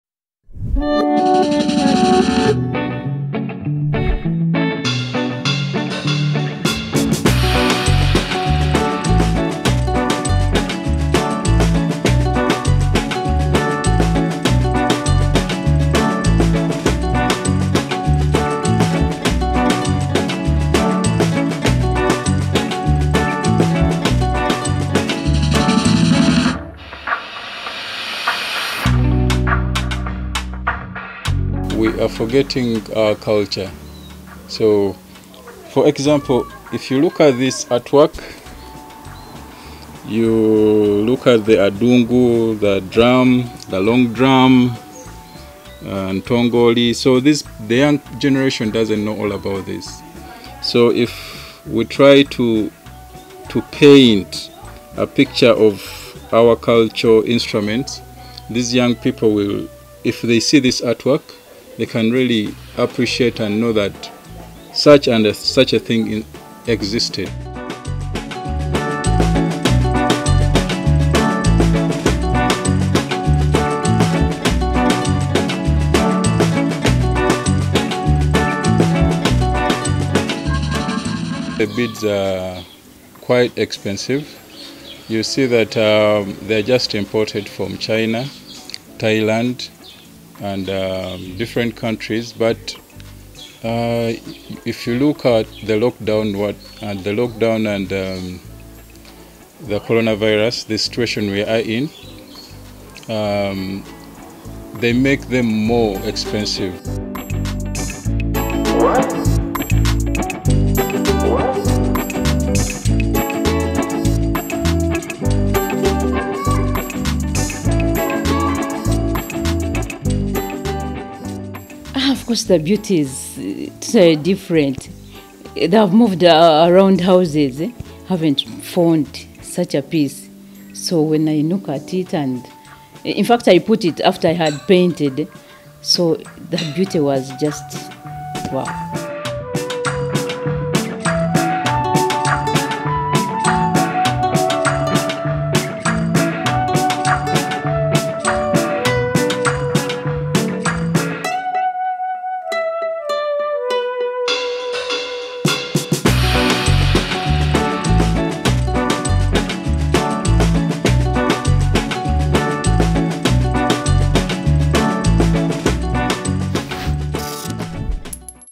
Uganda glass and bead Mosaic Artist
A Ugandan mosaic artist is hoping to preserve the country's cultural heritage and wildlife through his work.
Michael Ssozi designs mosaic art with glass waste and beads.